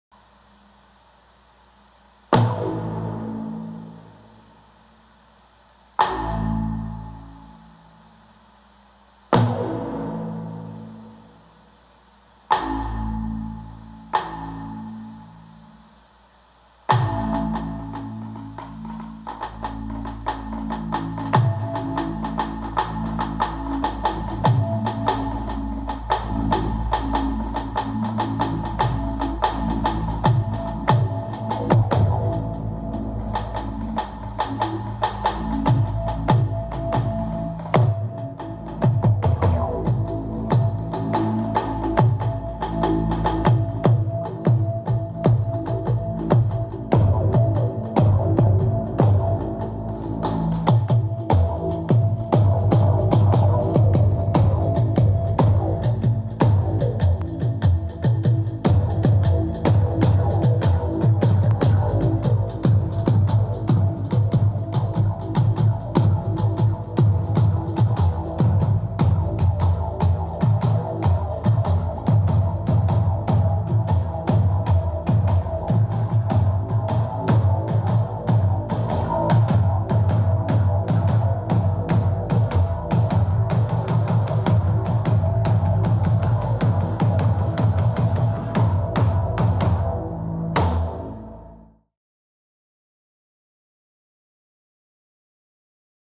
[카타디바이스] 5년의 결실...그것은 빛... 빛 장구!!!
5년의 개발기간, 그리고 완성~!
전통악기 장구와  디지털의 만남
스마트 장구~!!!
Smart Janggu
Development period of 5 years, and completion ~ !
The encounter of digital instruments with traditional instruments
Smart Janggu ~ !!!

빛을 연주하다!
Play the light!